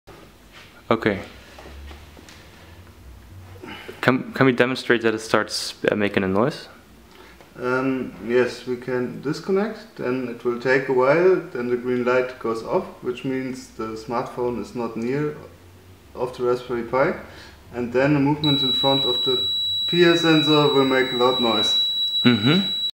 Raspberry Pi smartphone bluetooth alarm system
It is quite easy to setup an alarm system which detects a specific bluetooth device with a Raspberry Pi (3). The setup consists of a piezo buzzer, a LED and a passive infrared sensor. When the smartphone is not detected, waving a hand in front of the sensor causes the alarm to go off.

There was a small bug which causes to trigger the LED with an delay which is fixed in the tutorial.